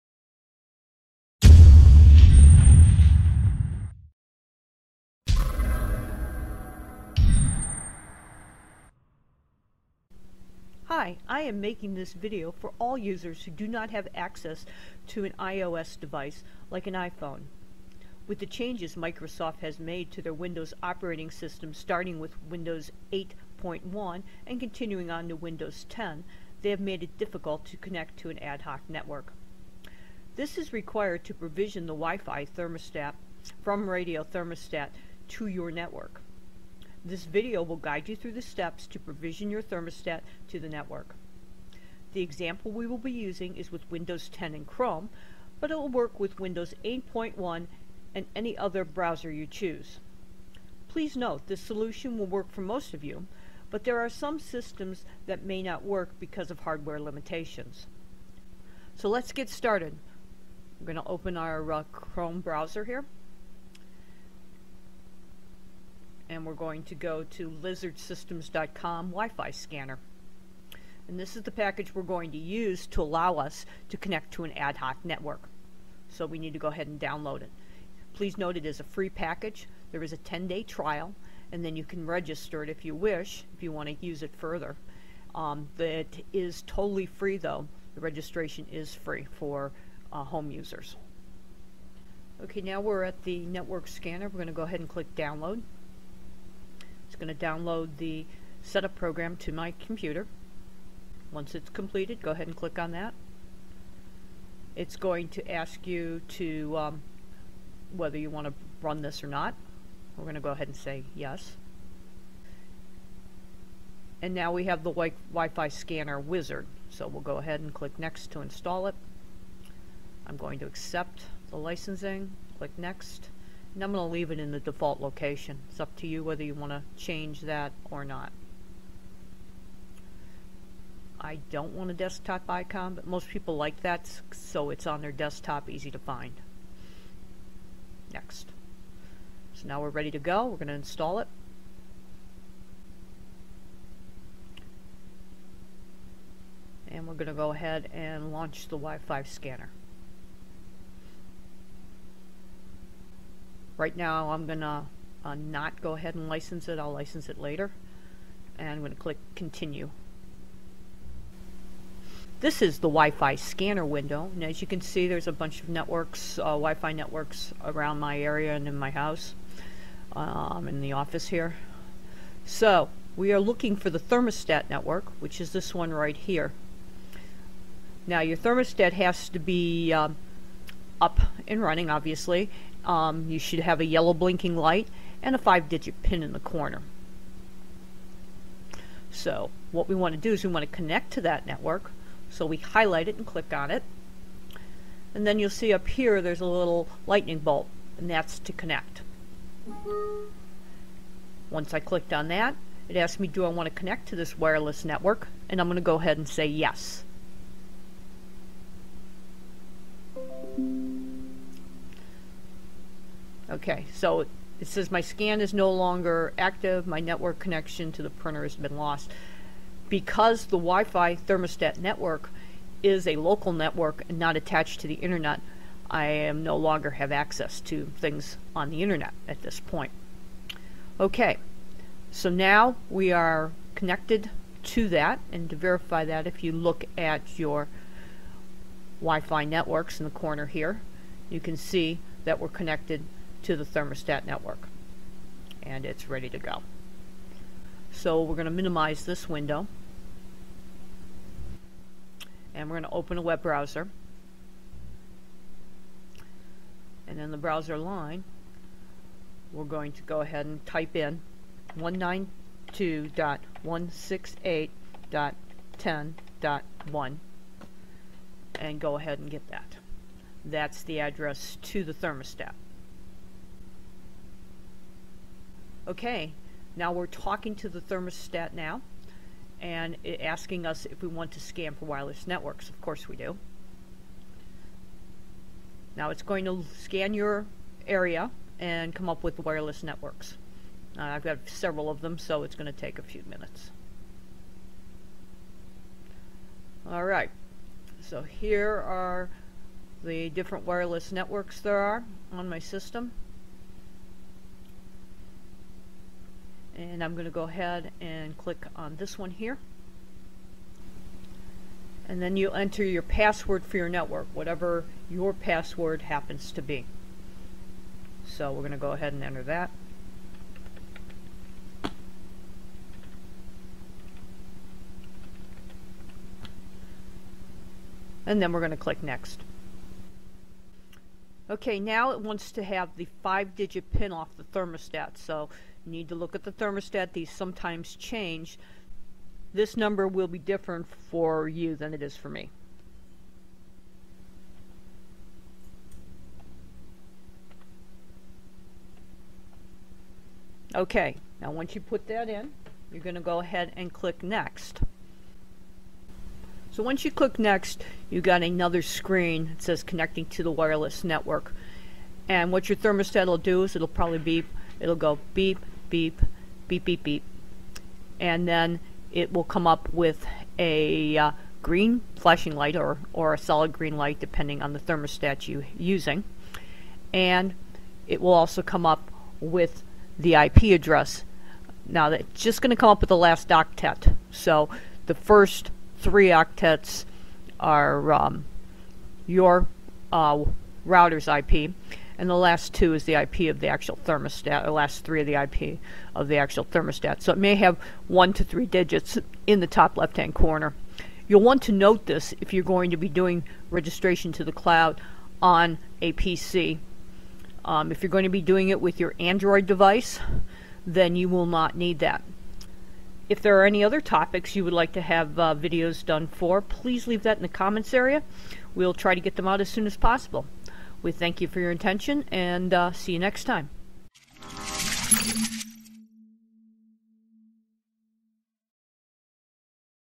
How to provision a RadioThermostat WiFi thermostat with Windows 10
In this video we will walk you through provisioning a WiFi RadioThermostat to your network with windows 10 or 8.1
This method is valid for the following thermostat models with a RTMV-01 WiFi module installed - thermostats included CT50, CT80, CT30, CT32 and 3M50